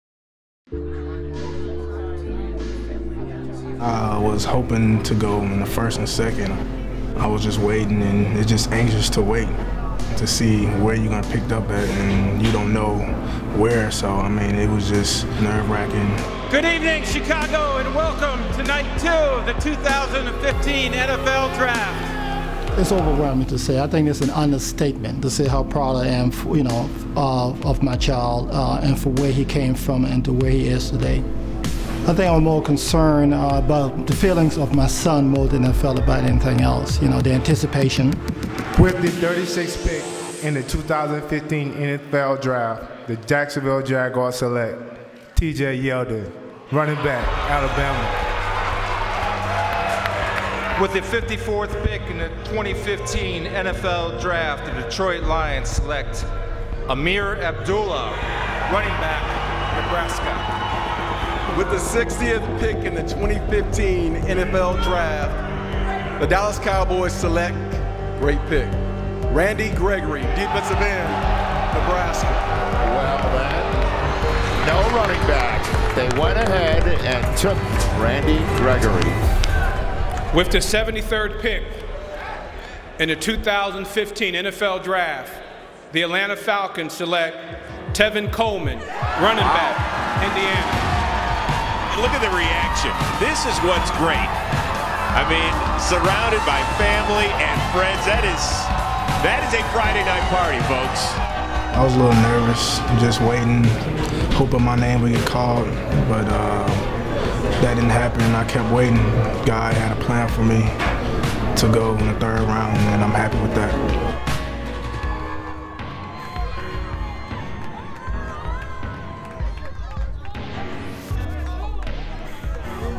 Tevin Coleman Draft Day Feature
See how Tevin and his family shared and celebrated the biggest moment of his life.

Produced by Corey Waller